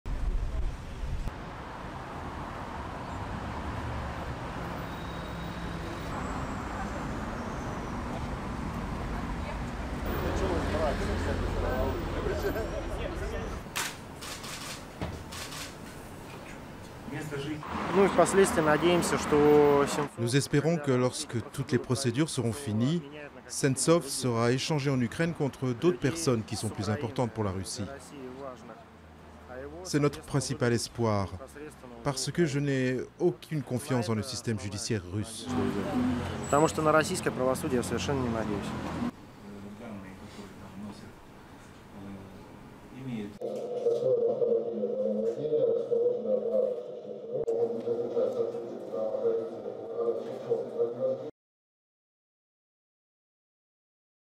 Russie: procès pour "terrorisme" du réalisateur Sentsov
Le procès du réalisateur ukrainien Oleg Sentsov, opposant à l'annexion de la Crimée par la Russie et détenu depuis plus d'un an à Moscou pour "terrorisme", a débuté mardi 21 juillet dans le sud de la Russie. Durée: 00:54